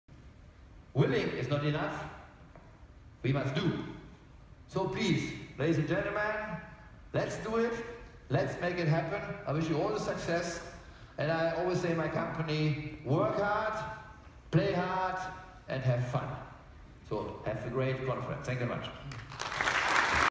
Statement zur ECOC 2016 in Düsseldorf, Germany
"The European Conference on Optical Communication (ECOC) is the largest conference on optical communication in Europe and one of the largest and most prestigious events in this field worldwide."

Dr. Bruno Jacobfeuerborn
CTO Deutsche Telekom AG and President of the VDE